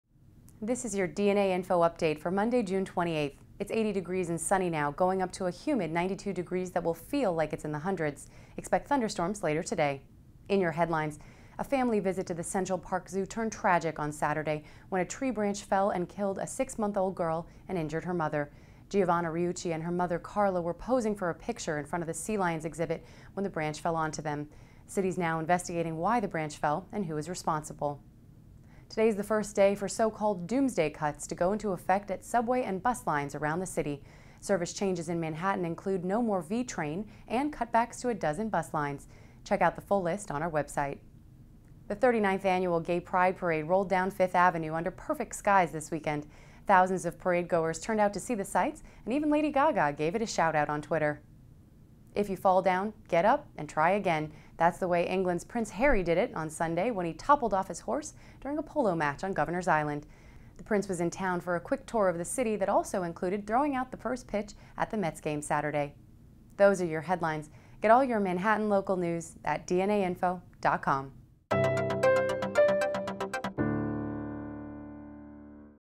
DNAinfo Manhattan News Update (June 28, 2010)
The city is investigating why a tree branch in the Central Park Zoo fell and killed a 6-month-old girl and injured her mother, today's the first day for "doomsday cuts" to go into effect at subway and bus lines around the city, the 39th annual gay pride parade rolled down 5th avenue under perfect skies this weekend, and England's Prince Harry toppled off his horse during a polo match on Governor's Island but got right back on.